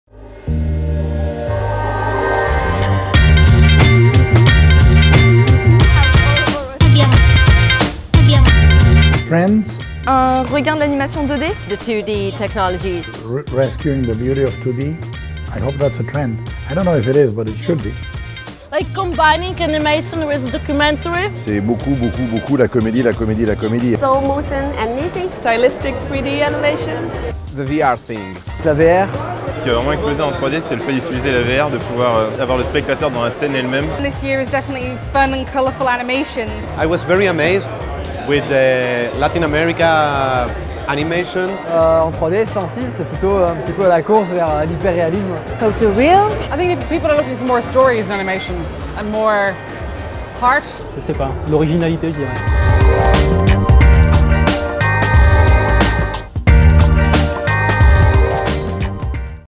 ICE CREAM TALKS - MIFA 15 JUIN 2017
Les ICE CREAM TALKS ? 1 jour = 1 question + 1 glace.
Du 13 au 16 juin 2017, ICE CREAM TALKS arpente les allées du Mifa pour recueillir avis, impressions et coups de cœur.
Les ICE CREAM TALKS? 1 day = 1 question + 1 ice cream.
From 13th to 16th June 2017, ICE CREAM TALKS wandering through the aisles of the Mifa to collect your opinions, impressions and memorable moments!